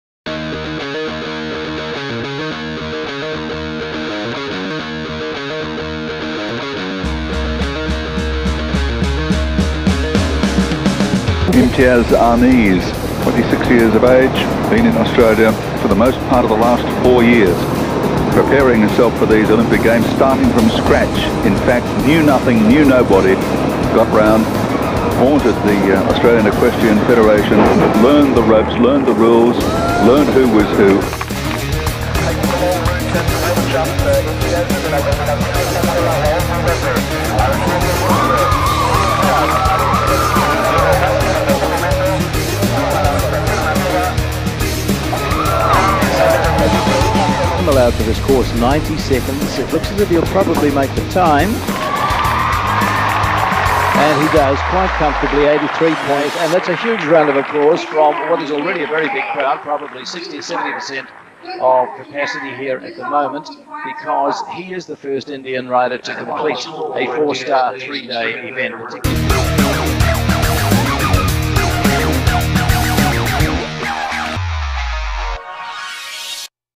Order Now - Riding Free : My Olympic Journey by Imtiaz Anees | HarperBroadcast
"In Riding Free, Imtiaz re-traces the major milestones of his riveting twenty-year-long journey. The stories he tells are heartfelt, emotional and inspirational for the next generation of dreamers-a way to ‘give back’, in small measure, the enormous goodwill and help he received from all kinds of people in his Olympics journey. Behind Imtiaz’s success are also the struggles and setbacks that pushed him to work harder and achieve peak performance. In a sport where the result depends on both man and animal, the deep bond Imtiaz shares with his horses will leave animal lovers spellbound. Here is a story that will inspire every athlete to ‘never give in’.